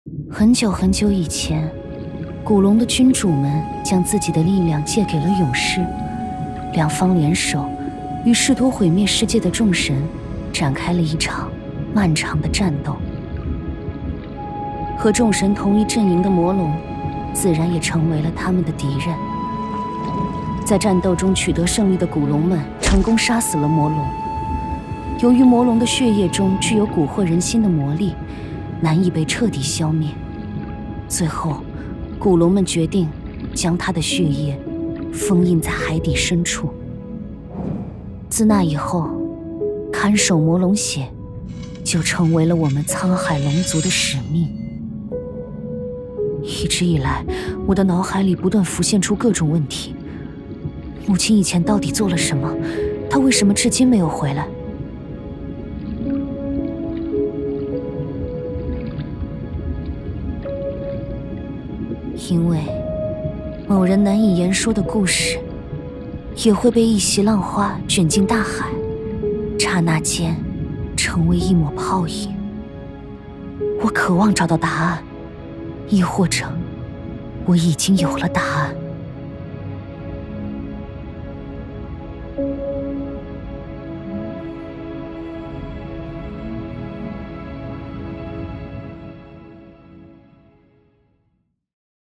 China 3rd Anniversary Character 'Yun' PV
This is the PV for the new character ‘Yun’, released for China's 3rd anniversary. -Publishing. BiliBili